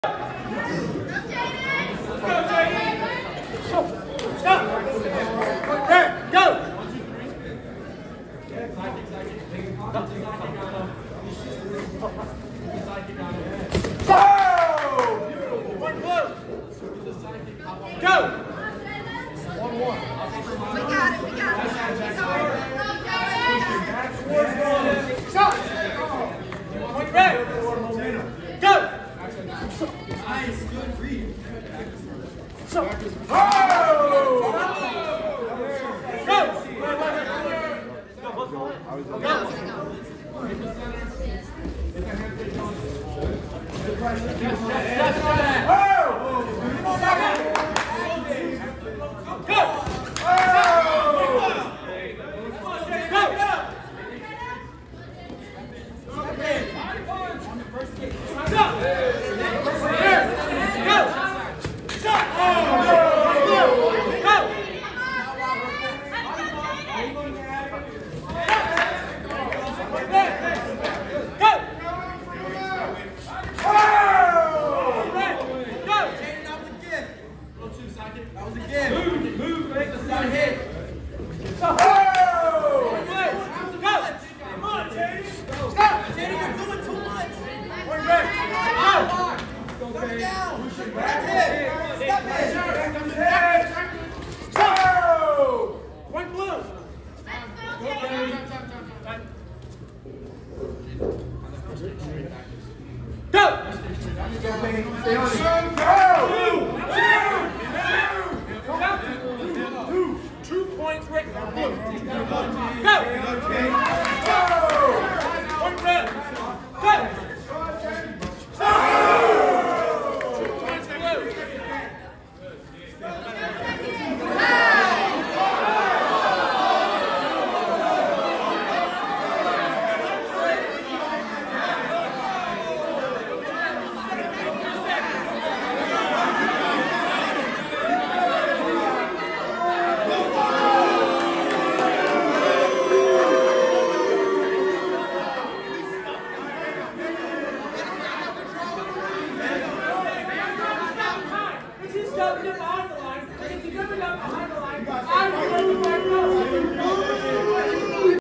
Kayne Talbott vs Jaydon Holland 15-17 Boys Open Weight - Diamond Nationals 2024
Kayne Talbott of Team All Stars and Jaydon Holland of Top Ten Team USA face off in the 15-17 Boys Open Weight sparring division at the 2024 Diamond Nationals in Minneapolis, Minnesota.

Special thanks to Mike Brumme for his contributions to documenting the events at the Diamond Nationals.